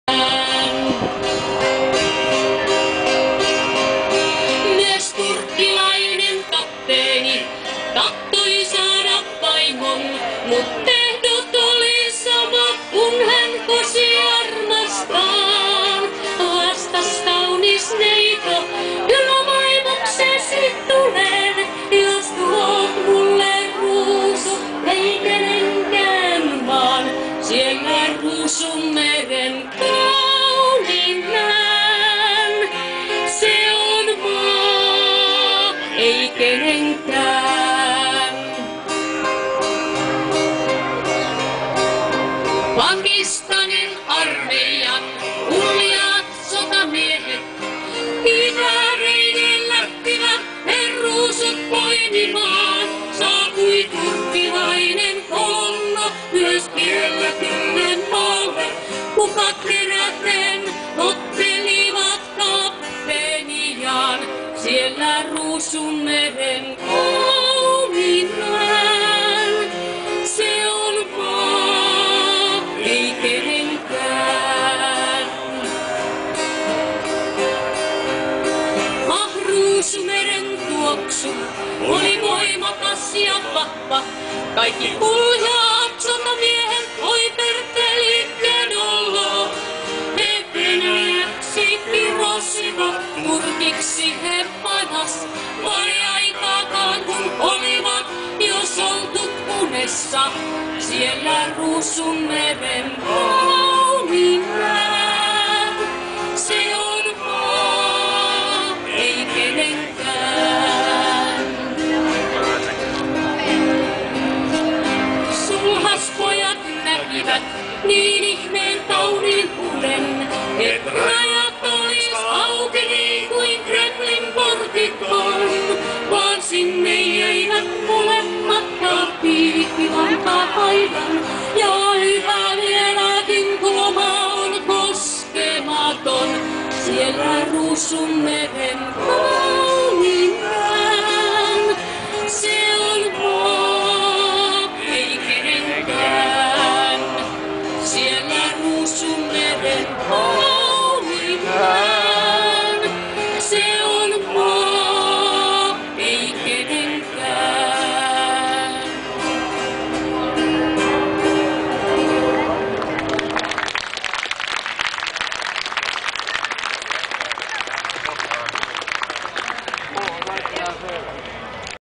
Comunistii finlandezi sarbatoresc ziua de 1 Mai
Manifestari comuniste cu ocazia zilei de 1 Mai, in Tampere, Finlanda.
In Finlanda nu exista restrictii nici pentru extrema stanga nici pentru extrema dreapta, libertatea de exprimare e absoluta.
Se poate defila si cu svastica si cu secera si ciocanul...